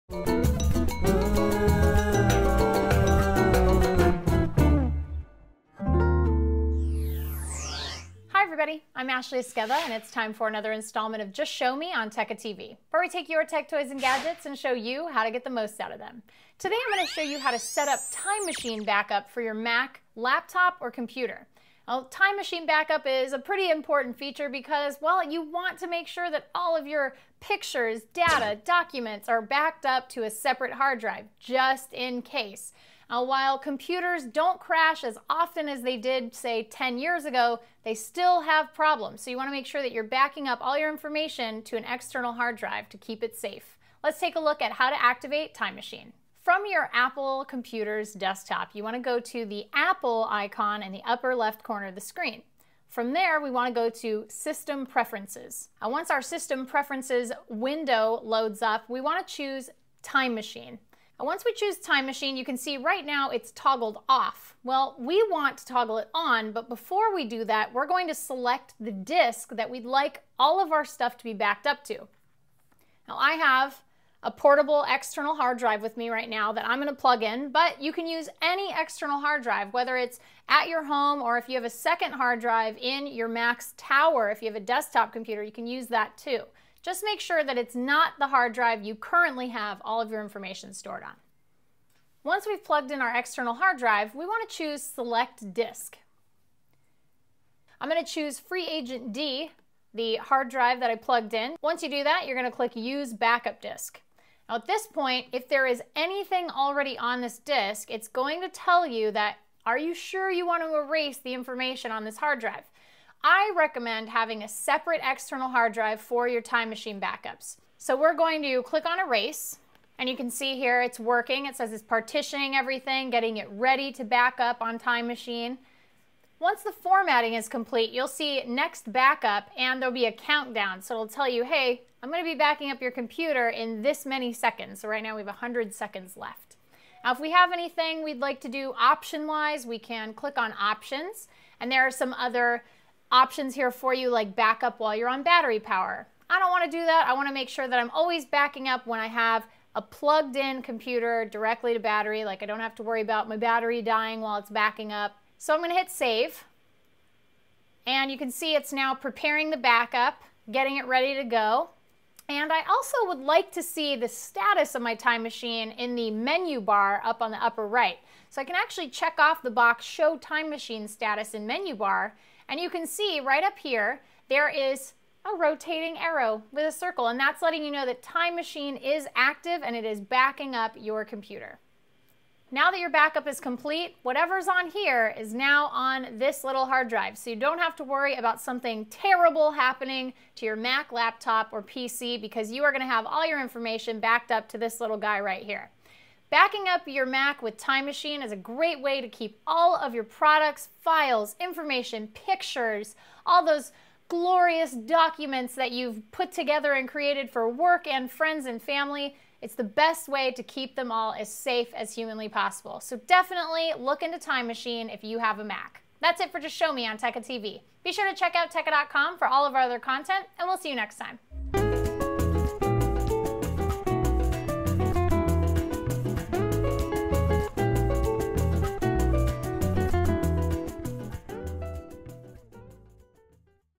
Just Show Me: How to back up your data on Mac OS X using Time Machine
We keep so many critical documents and precious memories on our computers that a hard drive crash can feel disastrous. If you use Time Machine on Mac OS X, that doesn't need to happen to you. We'll show you how to set up your backups, monitor its progress, and even customize your Time Machine options.